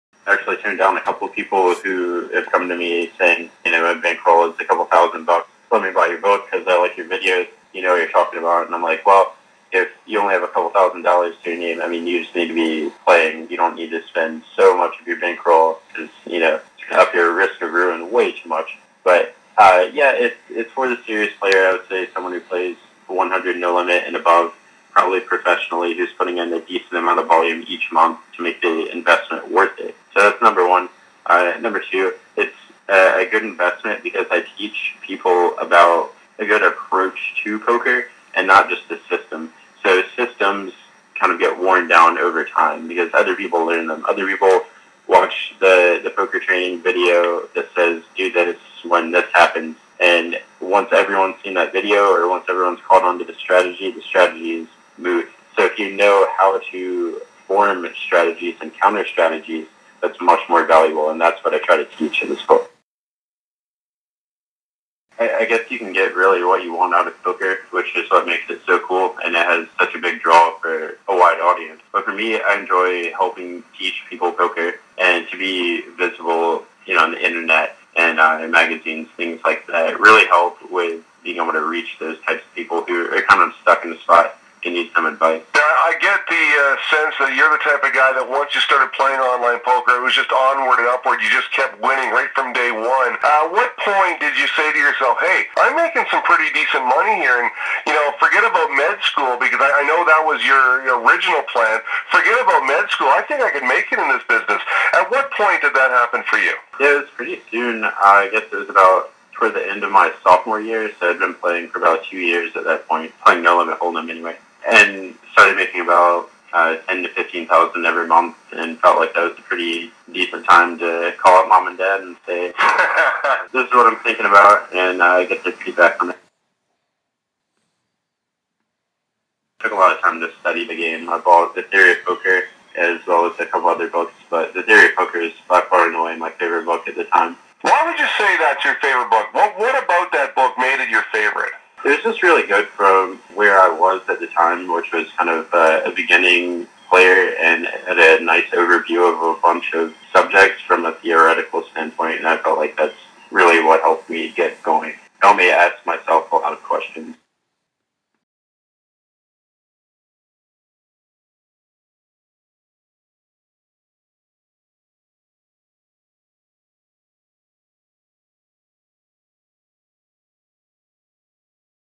The $1,000 Poker Book: "Not for Everyone," says Reid Young
Reid Young, professional poker player, author & instructor with Poker Sprout, discusses his Blue Boo: An Advanced Guide for No-Limit Hold'em Cash Games, his origins in poker & his favourite poker book.